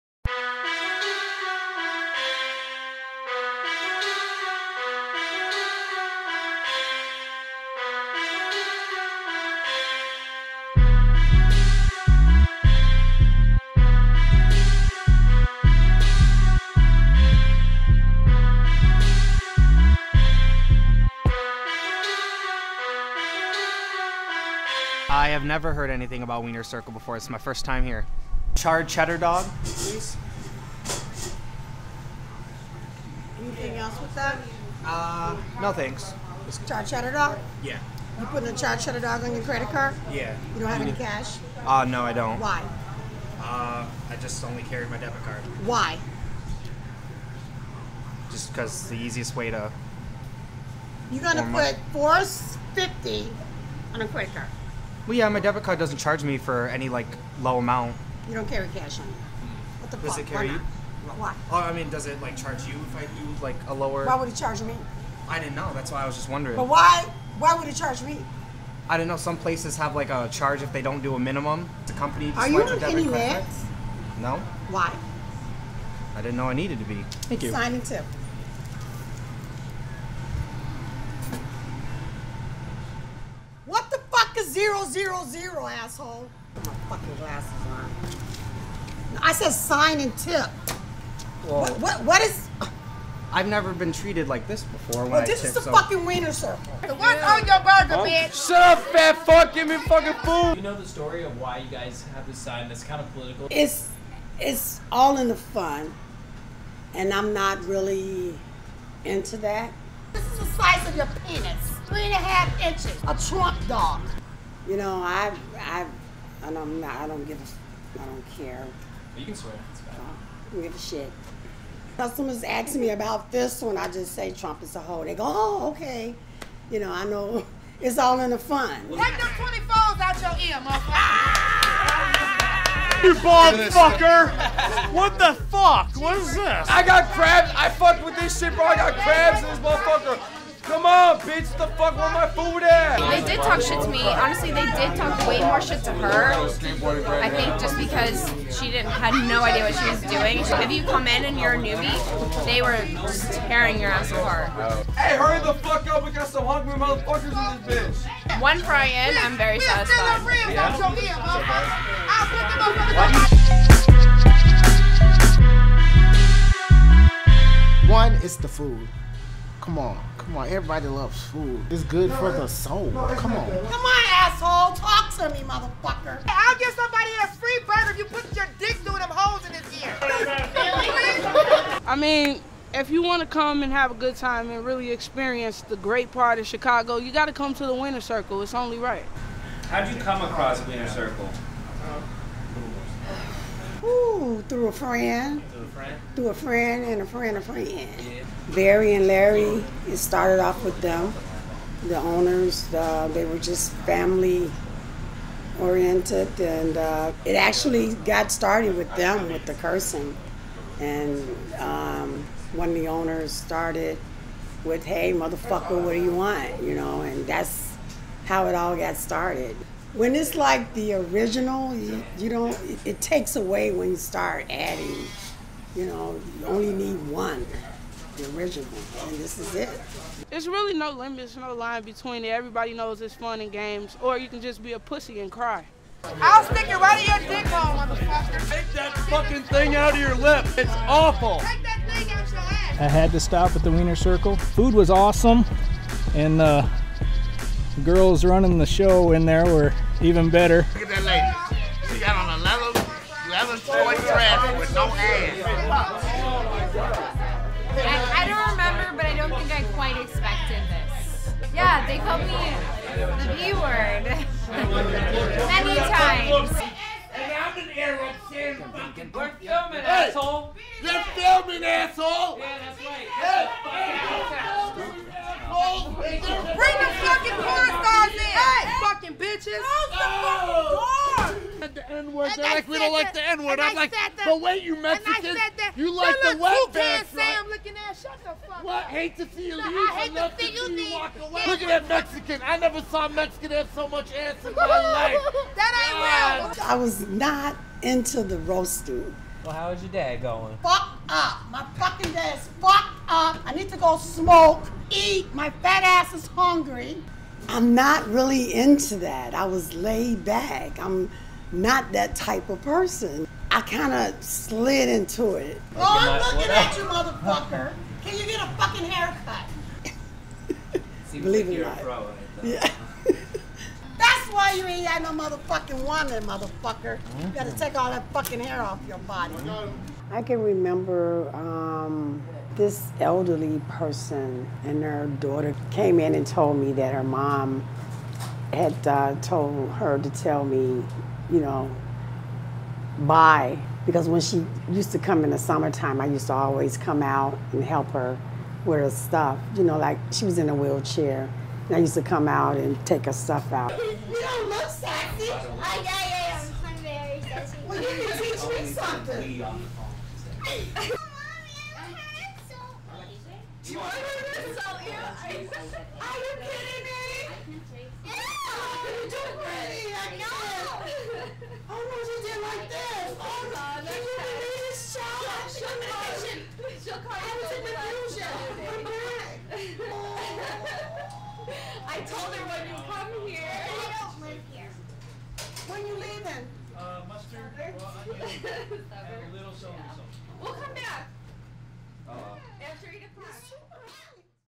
The Wiener's Circle Documentary
A short documentary exploring Chicago's greatest fast food establishment.

DePaul University, 2018